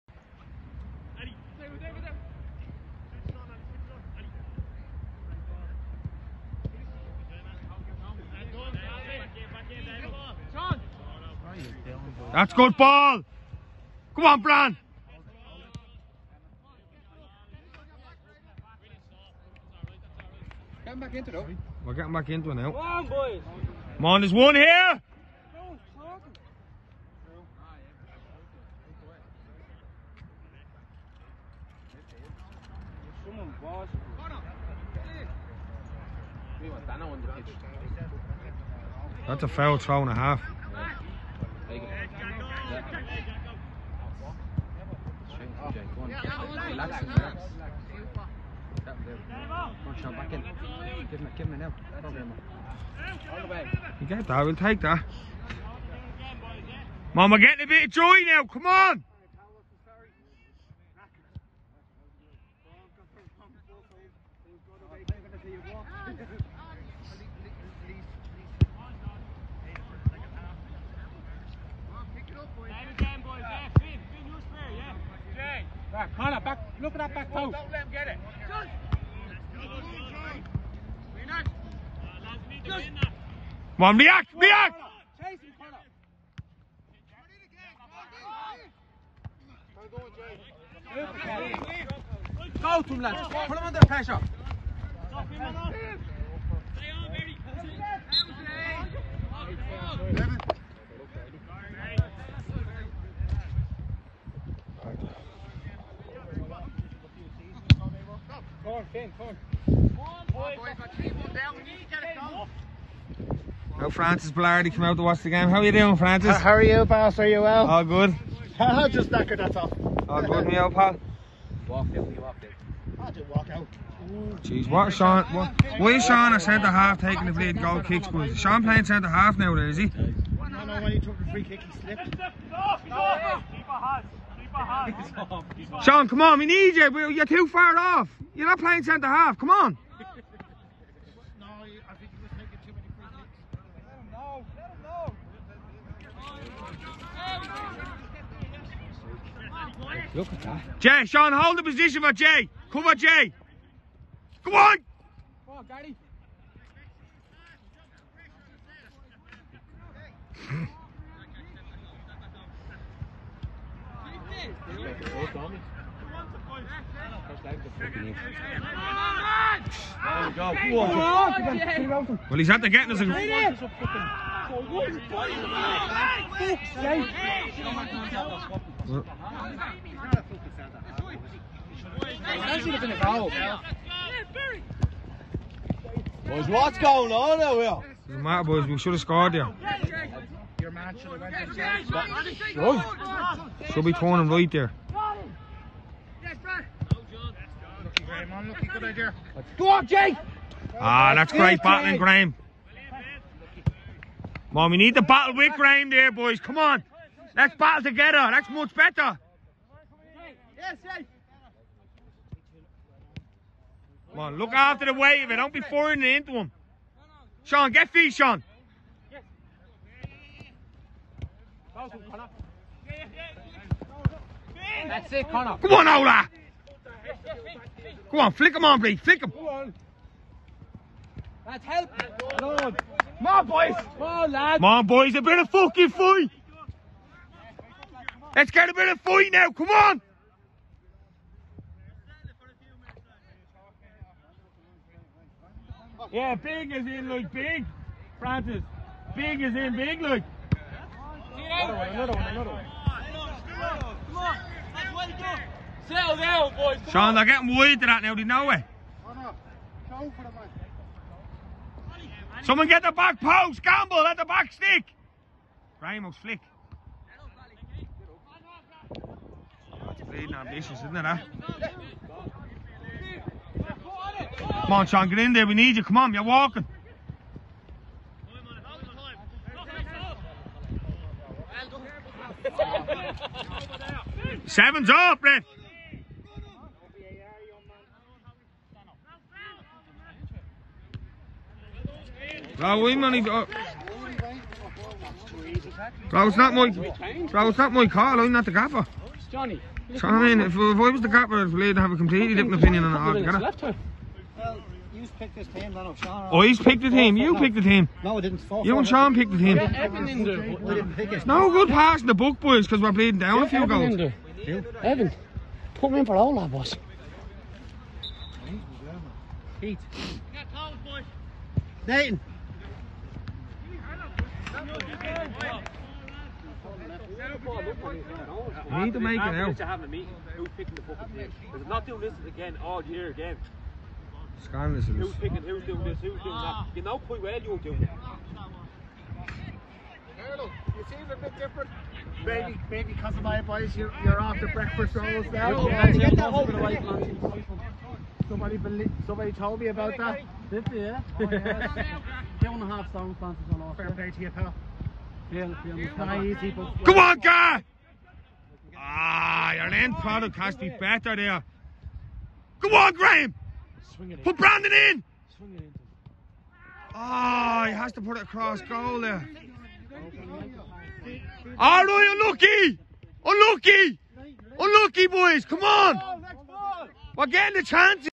Second half highlights from Tamhlacht FC Vs Drumcondra fc
Video from Dano